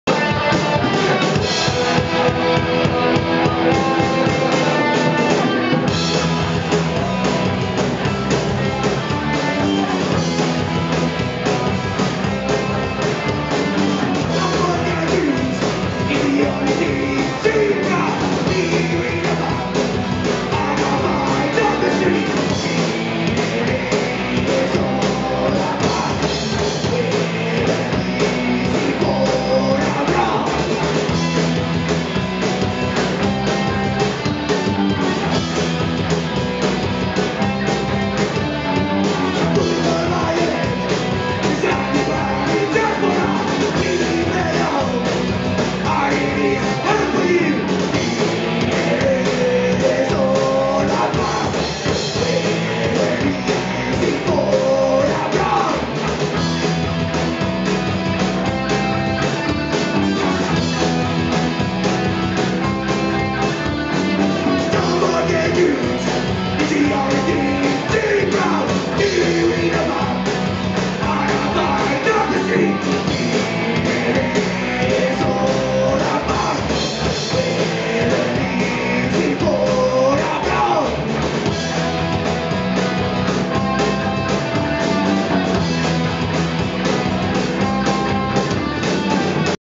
Gente Jodida CSO Proyecto Mayhem (11-05-13)
Concierto de apoyo a la caja de resistencia de Castellón con Gente Jodida (Oi! Castelló), Gargamboig (Hc-punk Borriol), Abuso de Poder (Oi! Castelló) y Zona Zero (Rap-metal vlc).